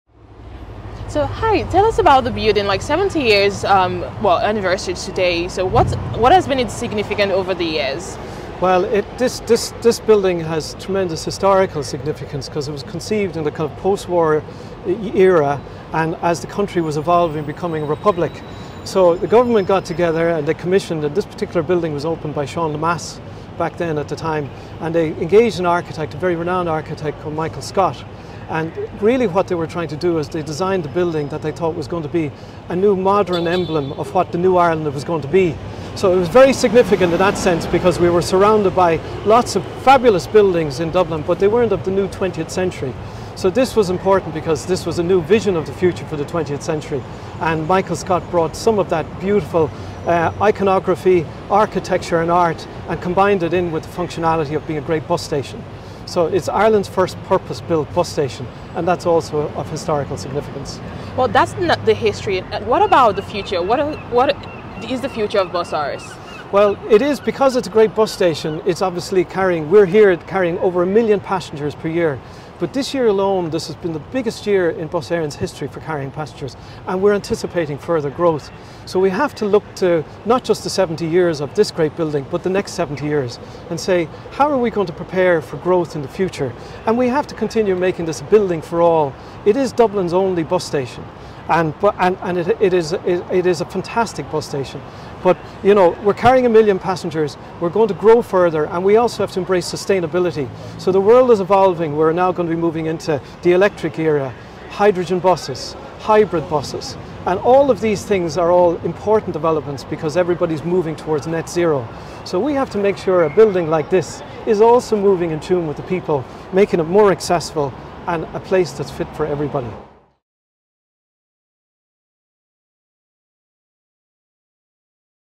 Bus Éireann celebrates Busáras 70 years of service
CEO of Bus Éireann speaks about the building's history, significances and plans for the future.